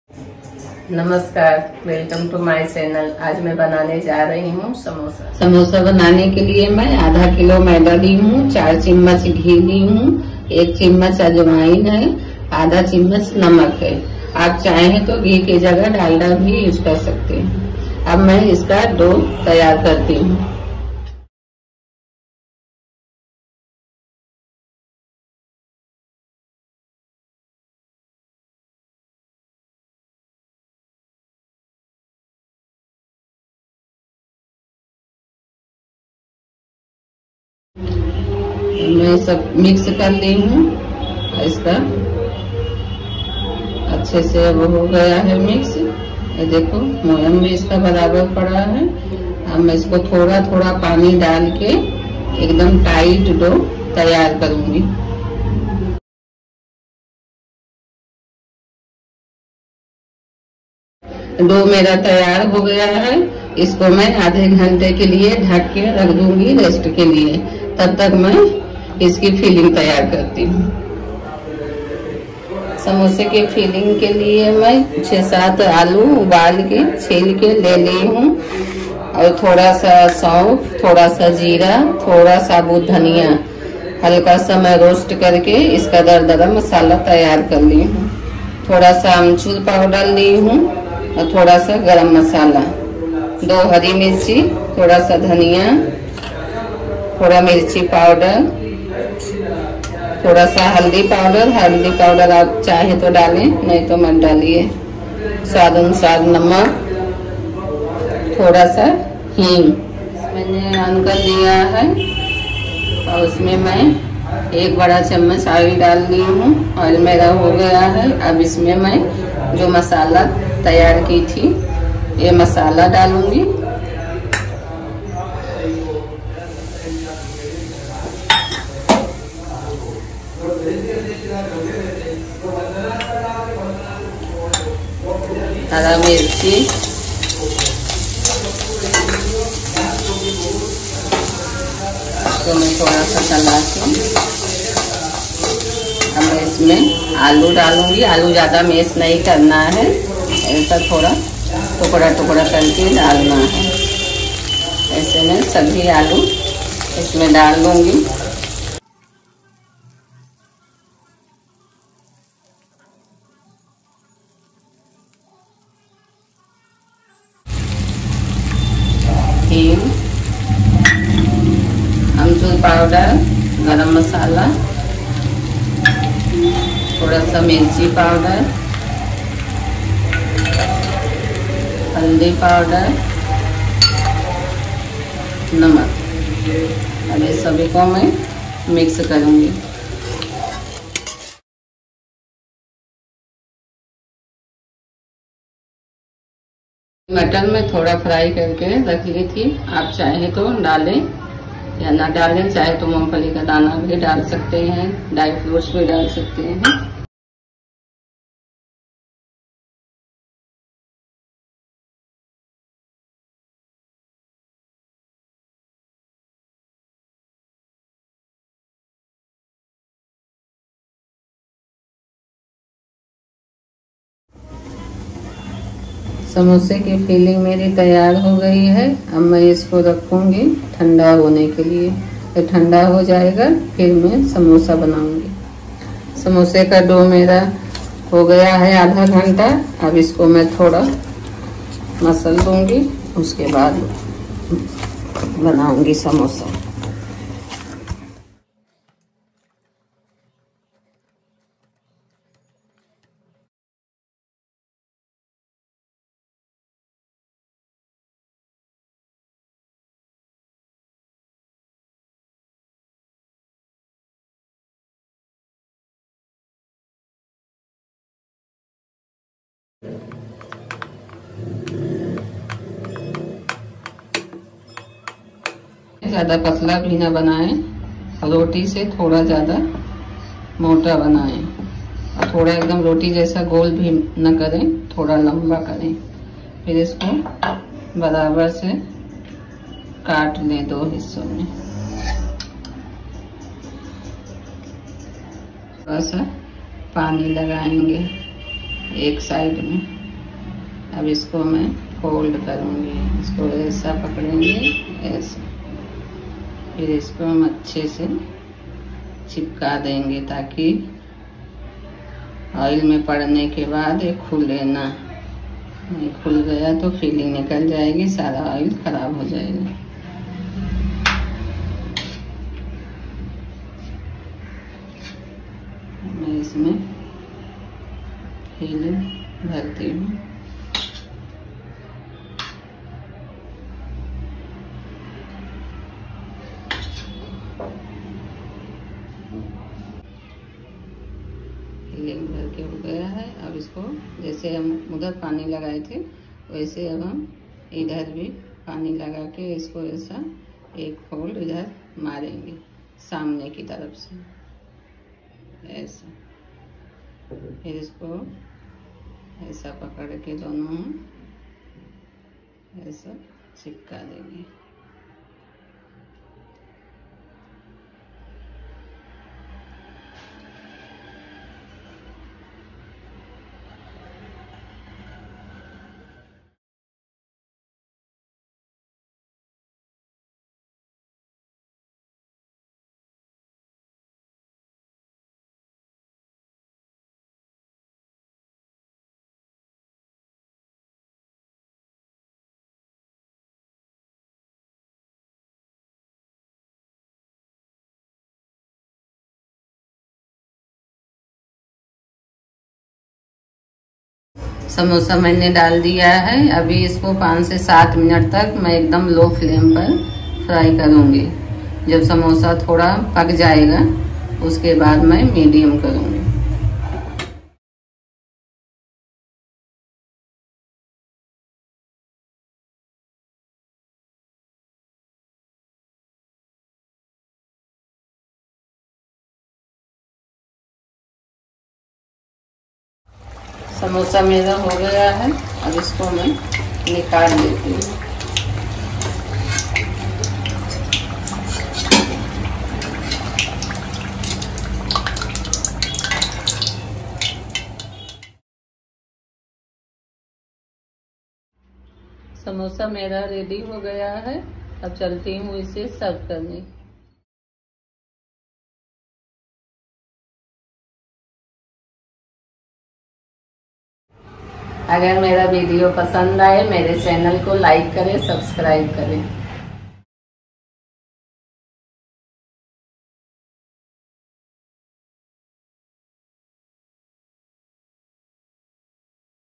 SAMOSA RECIPE | CRISPY SAMOSA | हलवाई स्टाइल समोसा रेसिपी
Hi friends, aaj mai halwai jaise samosa recipe share kar rhi hu .